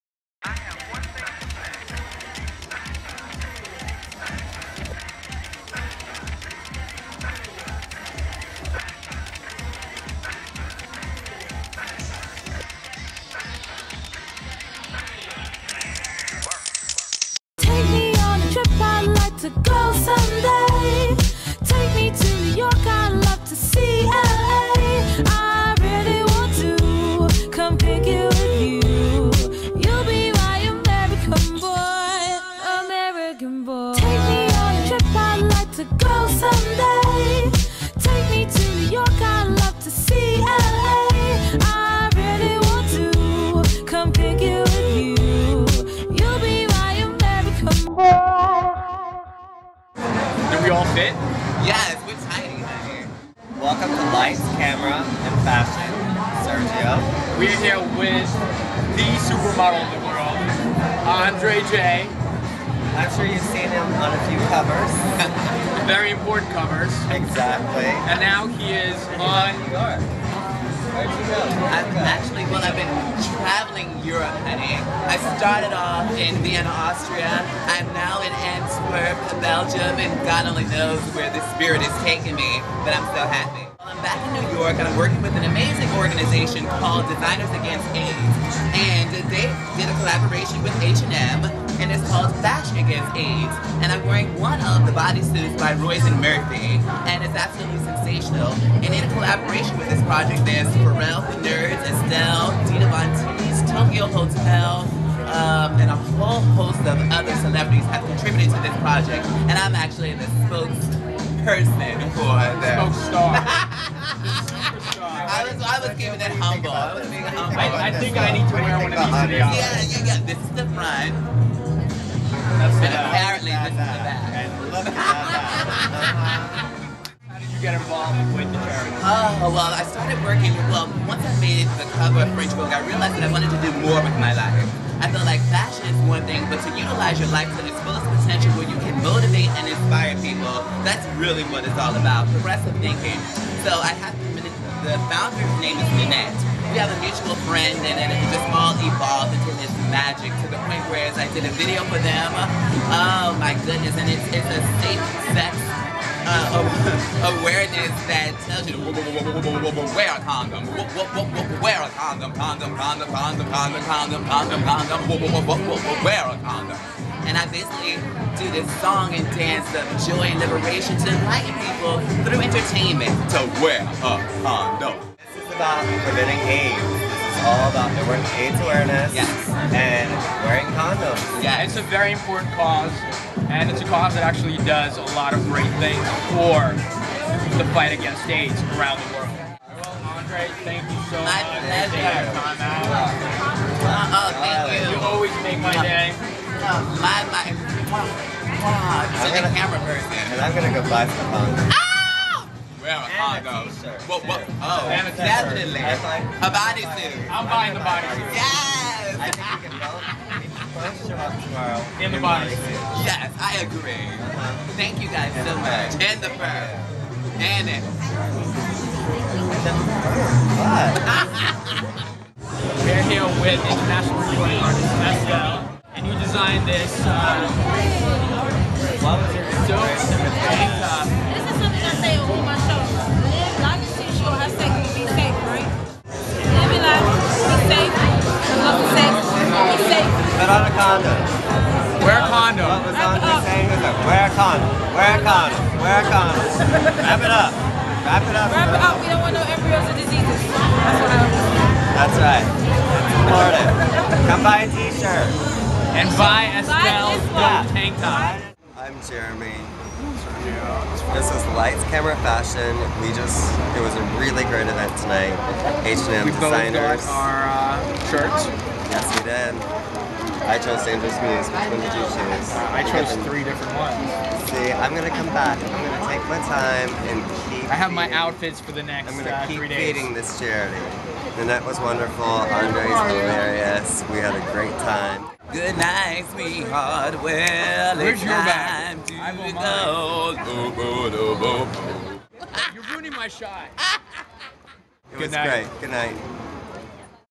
Lights Camera Fashion #3 with MarieClaire's Sergio & Jeremy
Sergio & Jeremy from Marie Claire and the Style Networks "Running in Heels" new video blog from NYC's hottest and exclusive events and parties.  This week Sergio and Jeremy attend the H&M 86th Street Store launch in NYC with a huge party for the new Designers Against AIDS campaign with interviews from Estelle and Andre J.